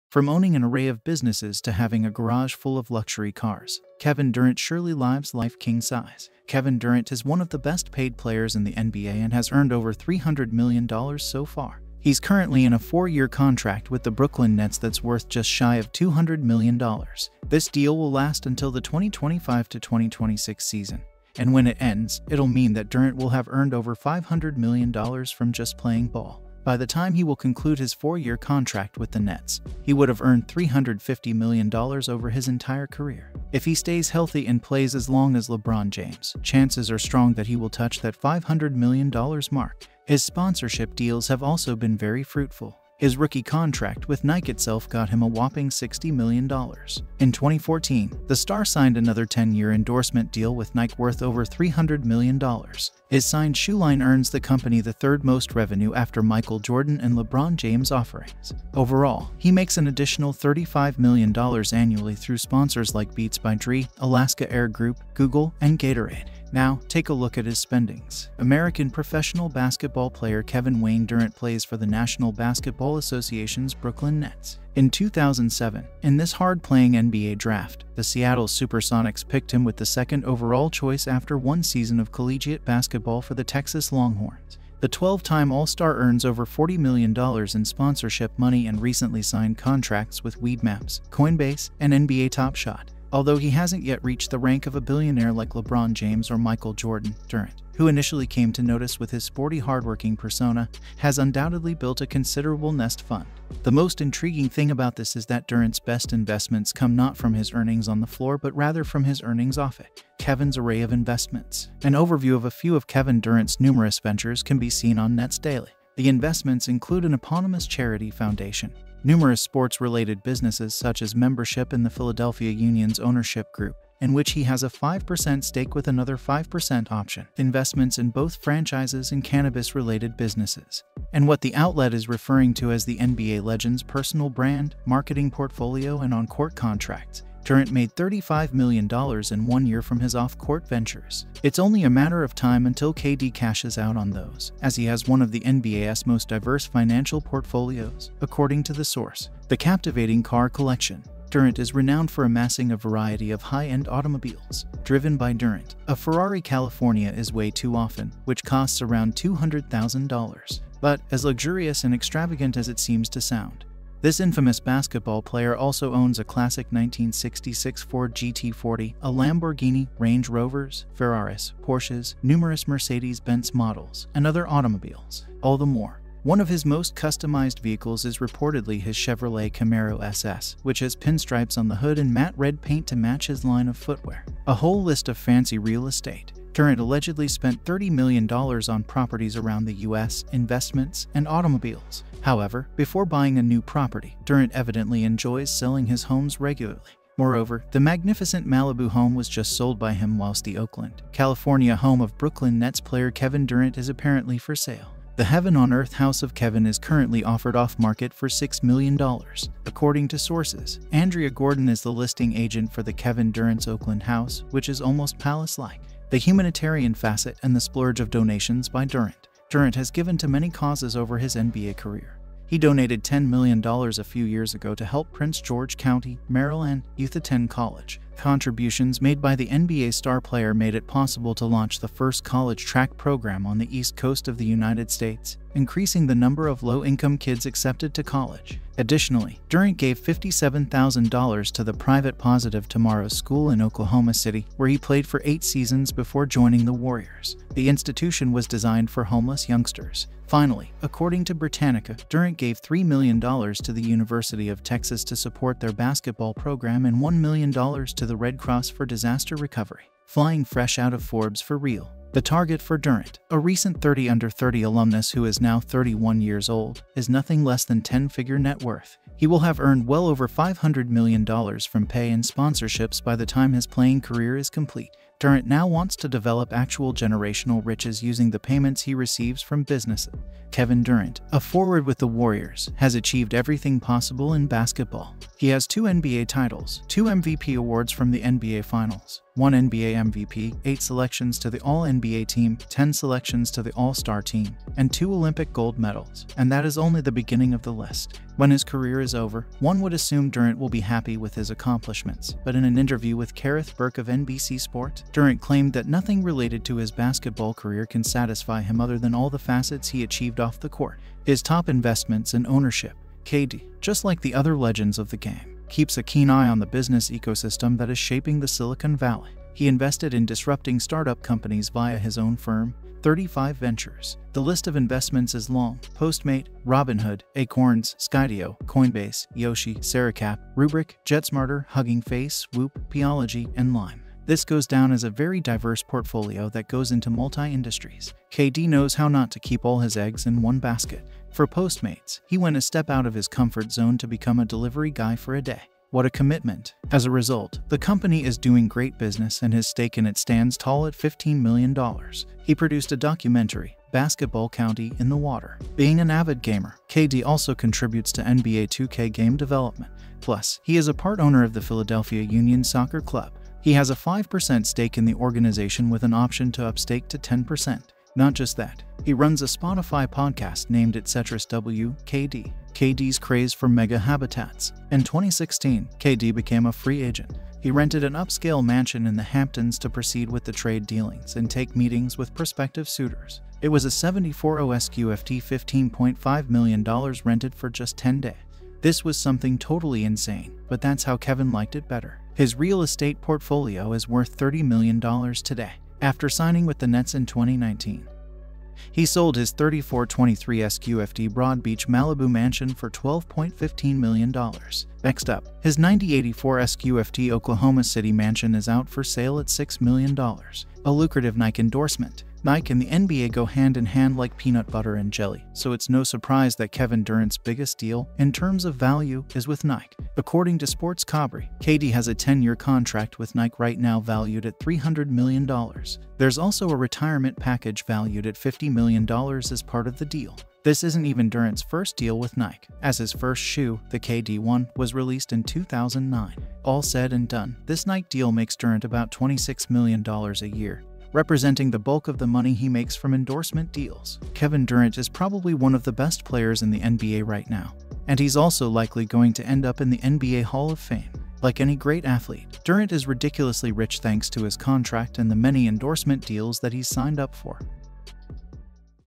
How Kevin Durant Spends his Money
This is How Kevin Durant Spends his Money ! What would you do with this type of money?! Let us know down below! How do you think the Nets will this season with all the Kyrie Irving?!